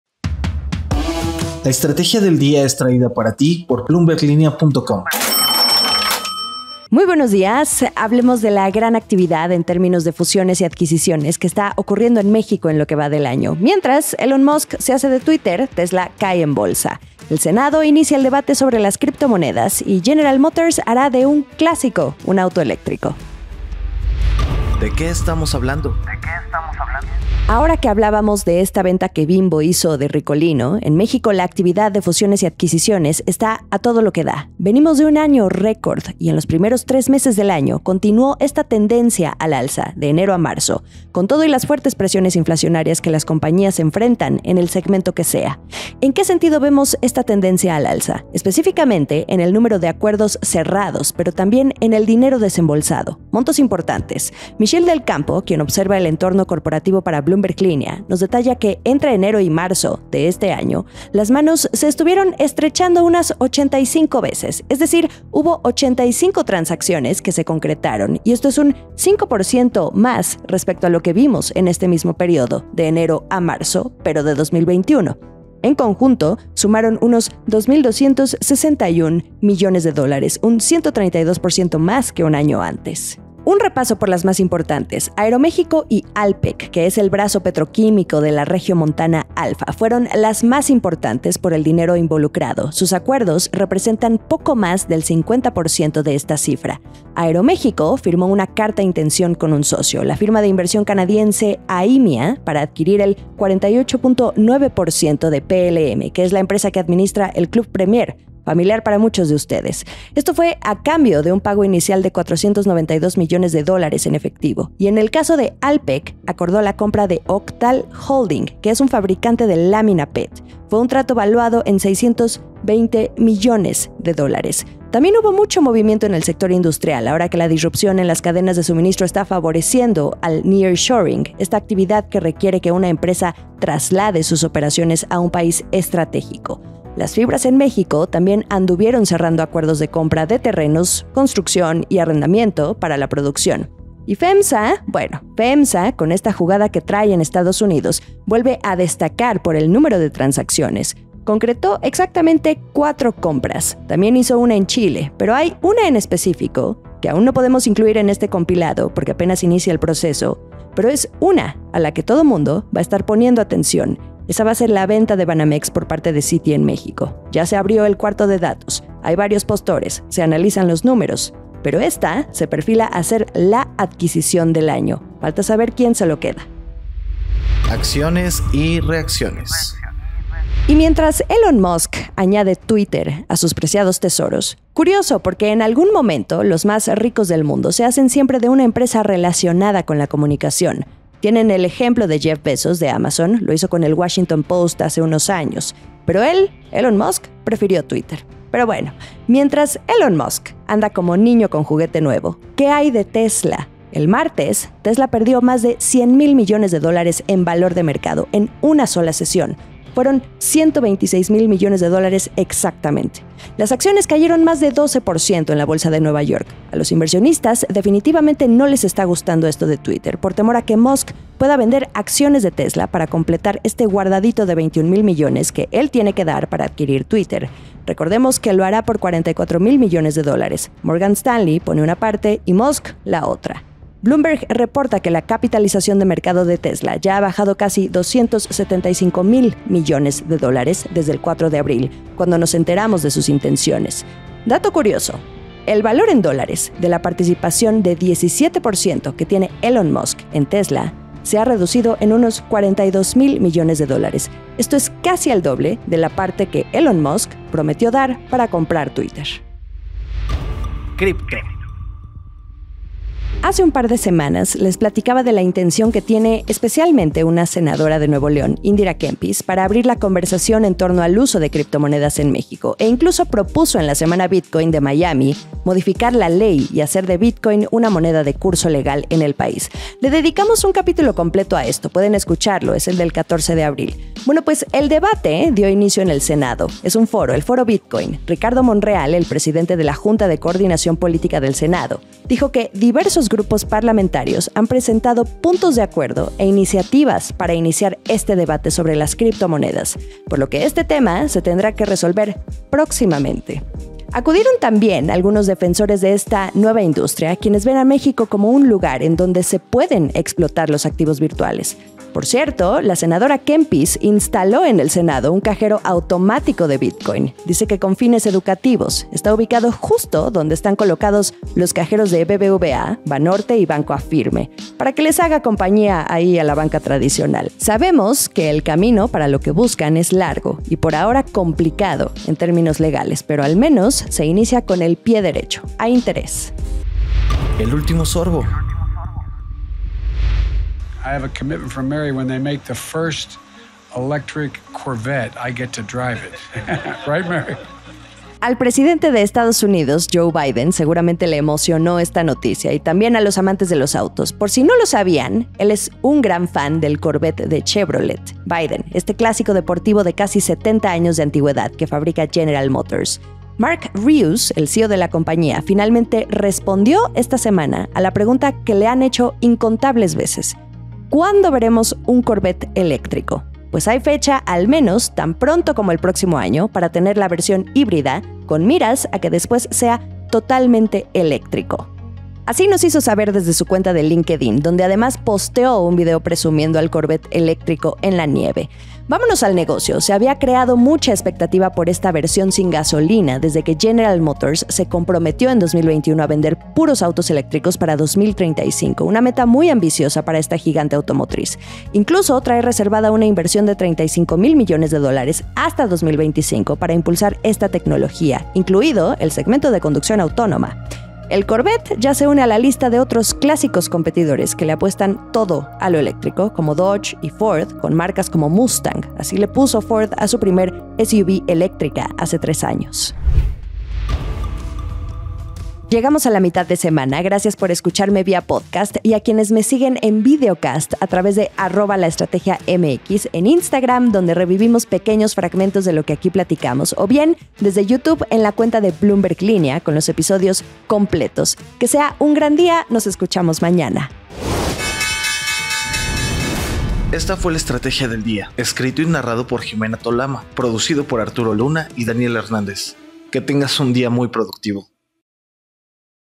Tesla pierde miles de millones en valor y viene el Corvette eléctrico
Hoy en LaEstrategiaDelDía hablamos de

Primero, un resumen de la buena racha en fusiones y adquisiciones que lleva México y también el Senado se abre al debate de las criptomonedas.

0:00 - Intro
0:25 - Fusiones y adquisiciones en México continúan tendencia al alza
2:57 - Tesla pierde más de US$100.000 millones en valor de mercado
4:20 - Senado inicia debate sobre uso y regulación de criptomonedas
5:40 - GM comenzará a fabricar modelos Corvette eléctricos